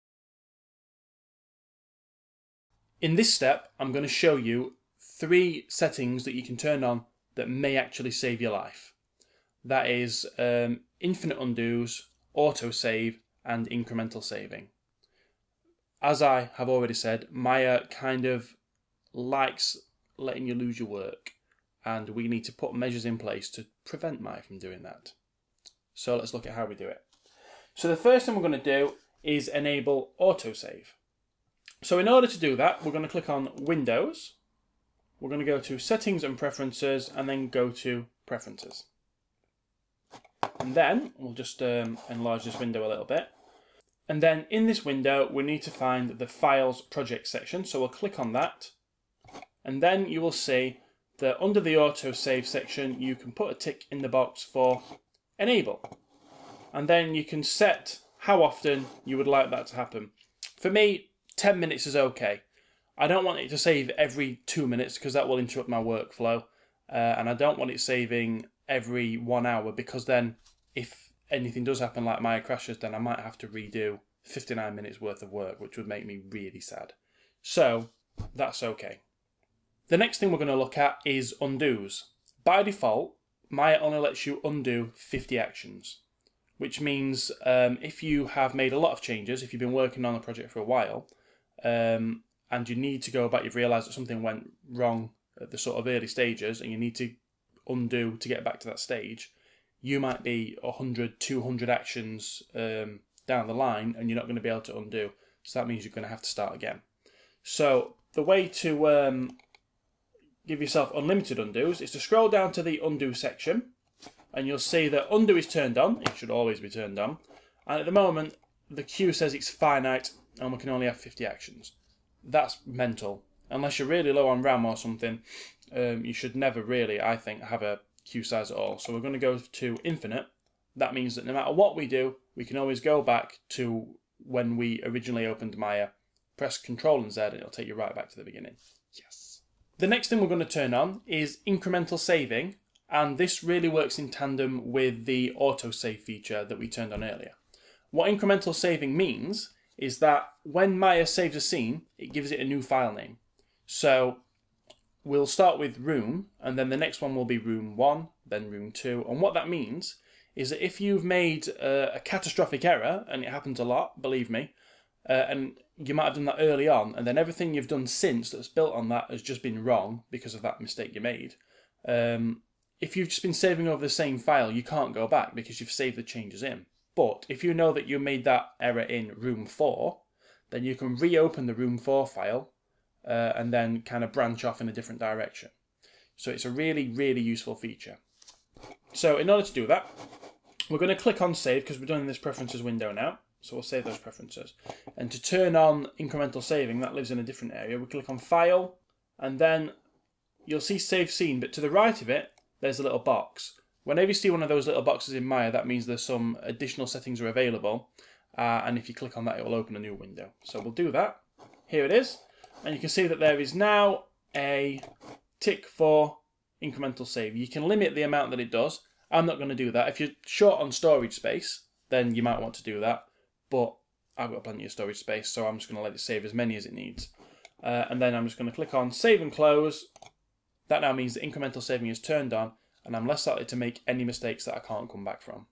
#4 The 3 most important settings in Maya!
Auto-save! Incremental saving! Infinite undo! This video shows you how to set all of these things up in Maya 2016. If you're going to be using Maya then you really need to get these set up properly to save you tears and self harm later.

You will need a copy of Maya 2016 in order to complete this tutorial.